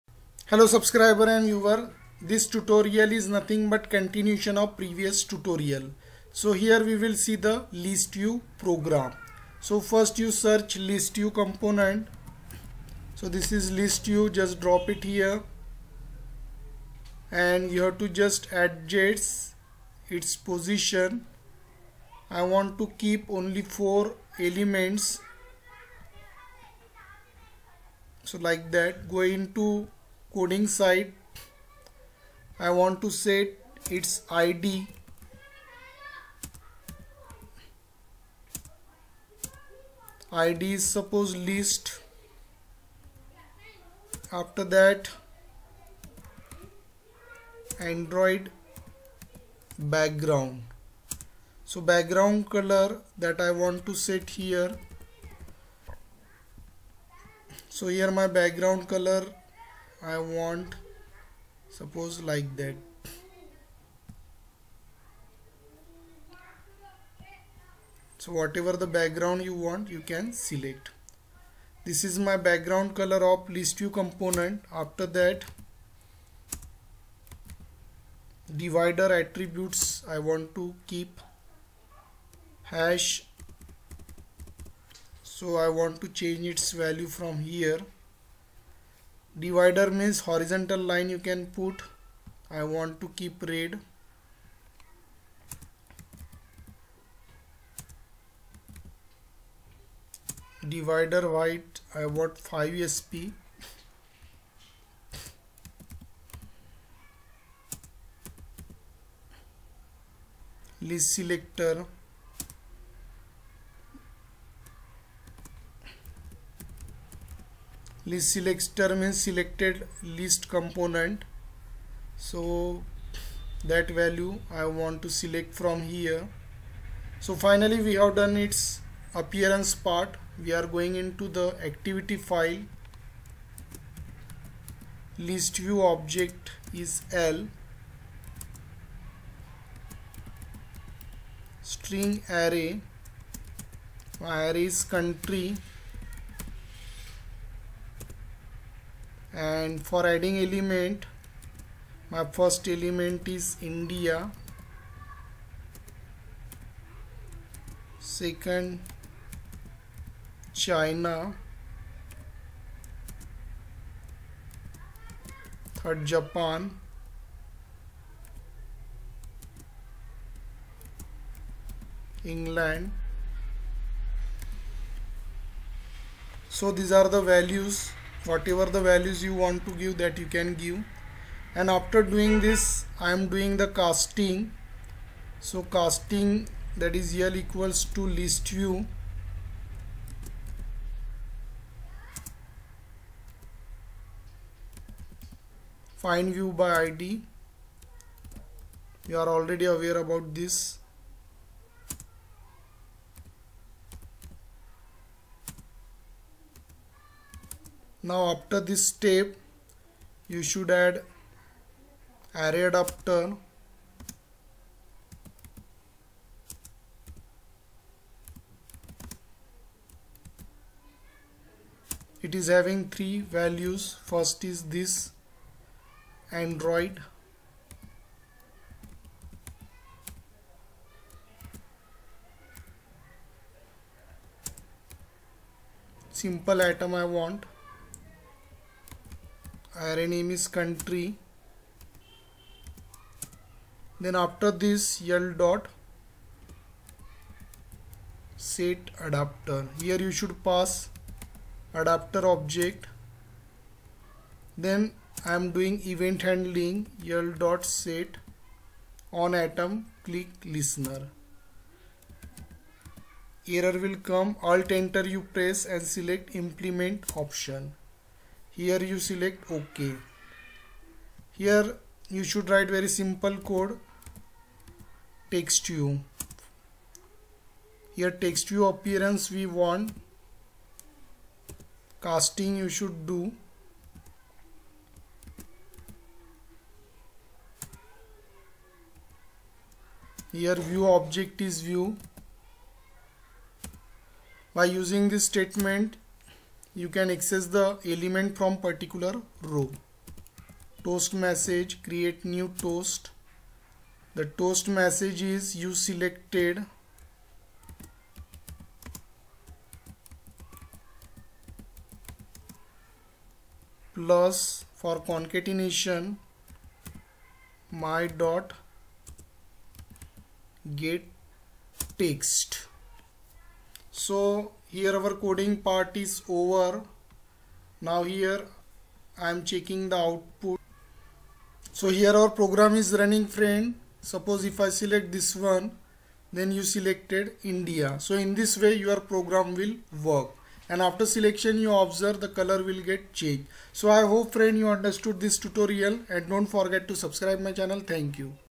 List View Program in Android,#49
In this Tutorial you will see a program of ListView in Android ,In coming tutorial we will see some more functionality of list view in Android.